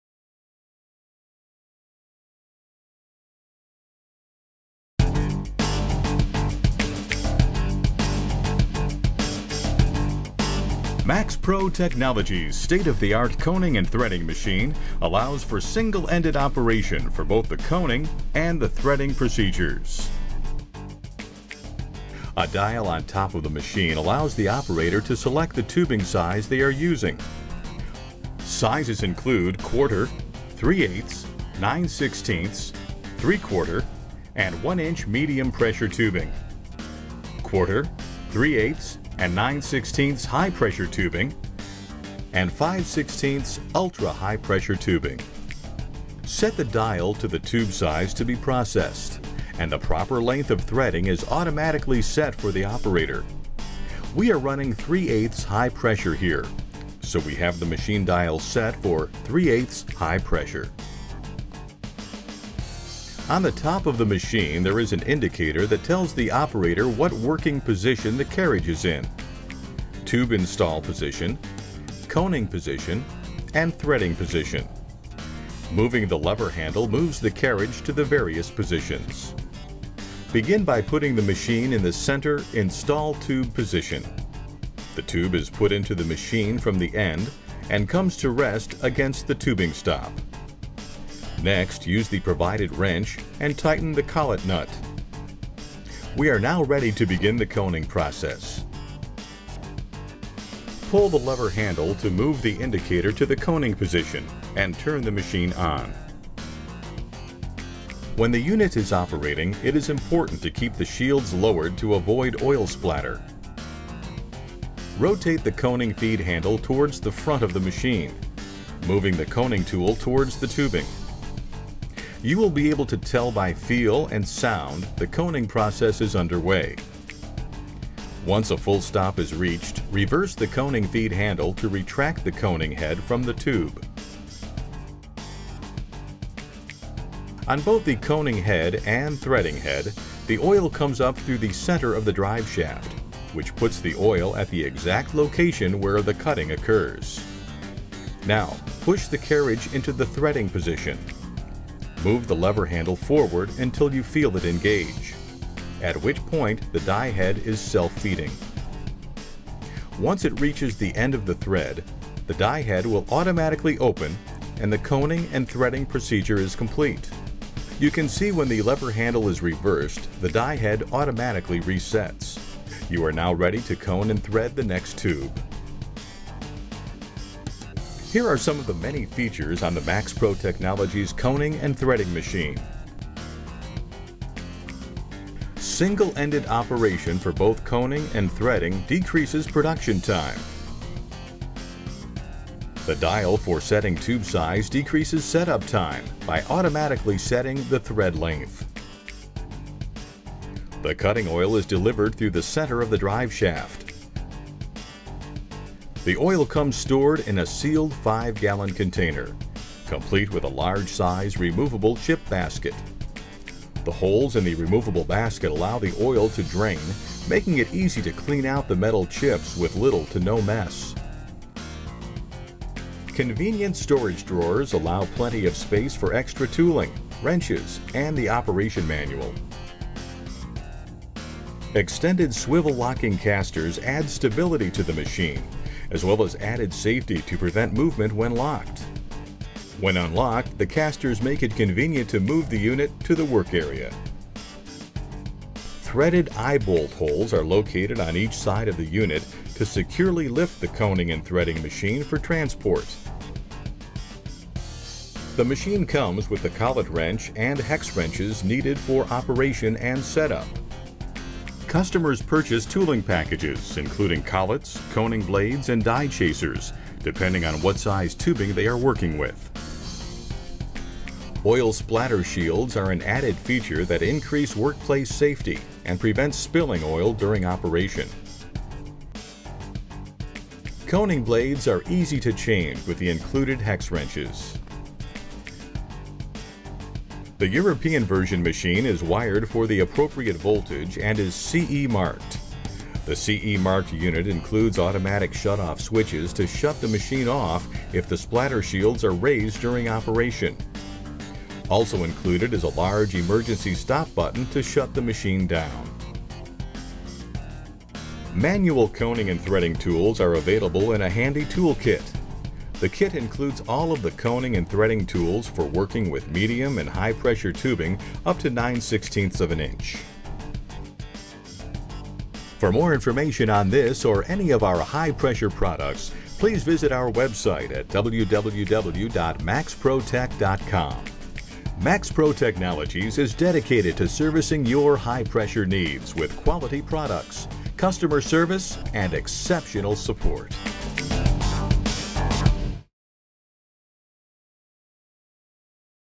Coning and Threading Machine at Maximator Australia
With this machine you can cone and thread all sizes MP and HP tube.

In stock ready to ship.

Purchase of hire!